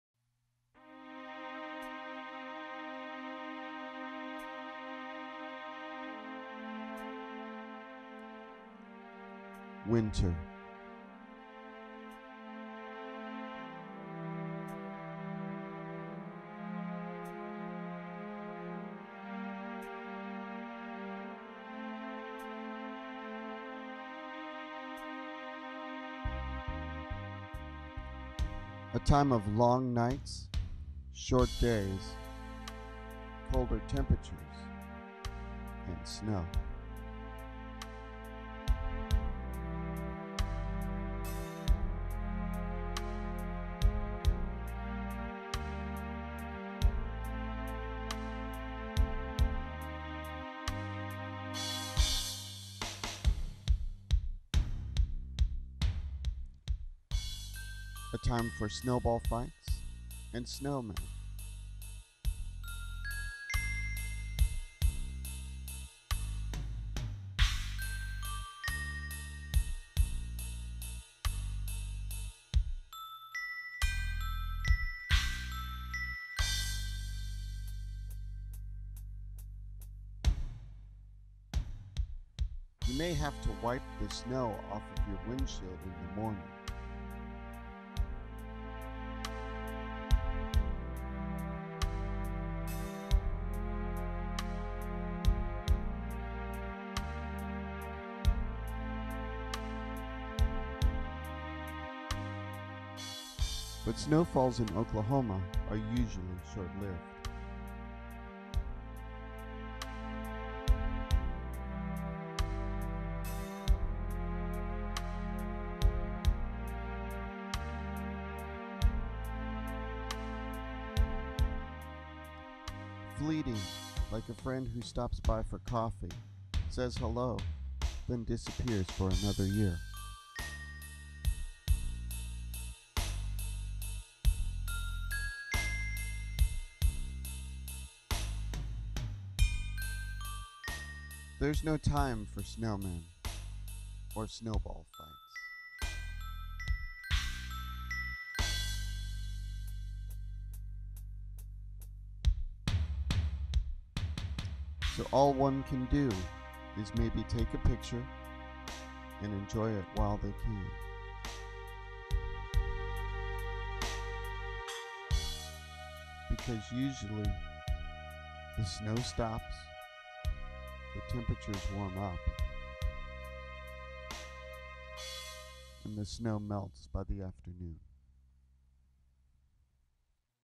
Snowfalls In Oklahoma
Winter
A time of long nights, short days, colder temperatures, and snow.
A time for snowball fights and snowmen.
You may have to wipe the snow off of your windshield in the morning.
But snowfalls in Oklahoma are usually short-lived.
Fleeting, like a friend who stops by for coffee, says hello, then disappears for another year.
There's no time for snowmen or snowball fights.
All one can do is maybe take a pictue, and enjoy it while they can.
Because usually, the snow stops, the temperatures warm up, and the snow melts by the afternooon.